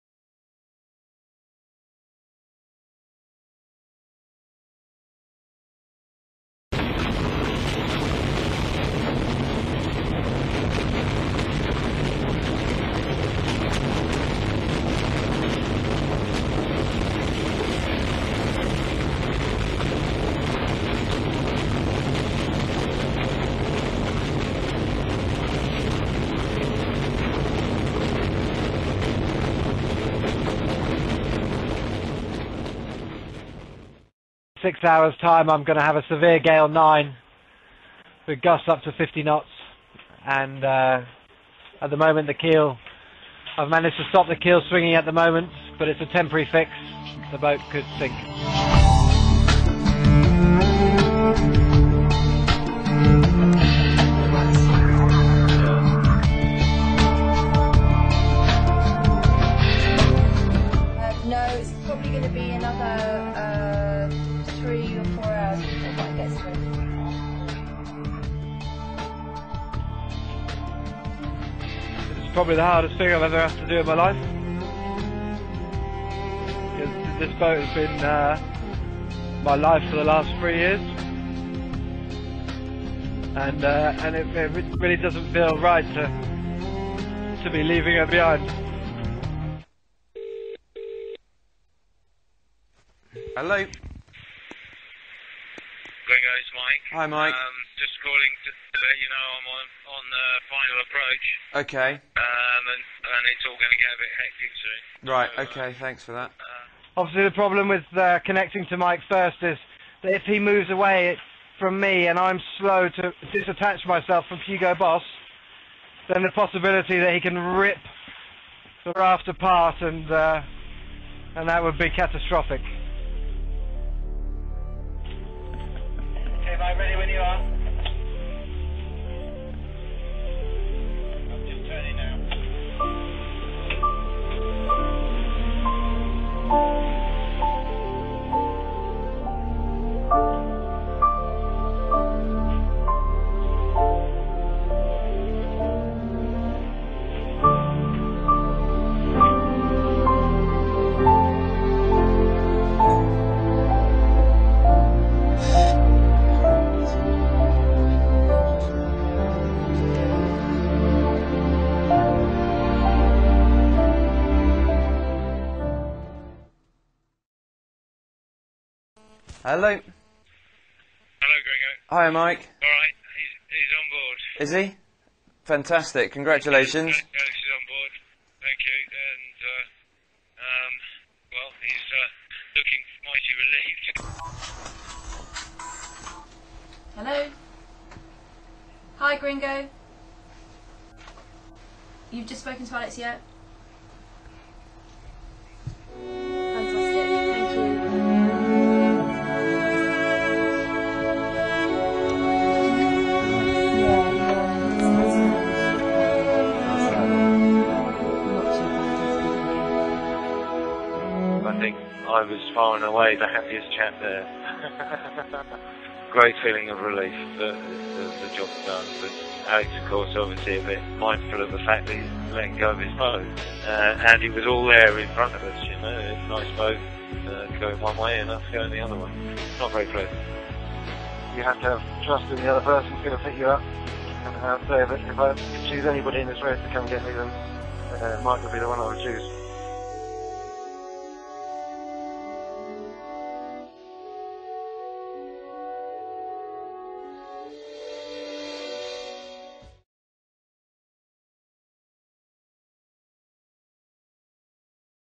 Belux 5 Oceans
Belux 5 Oceans race 2006 featuring Alex Thomson and Mike Golding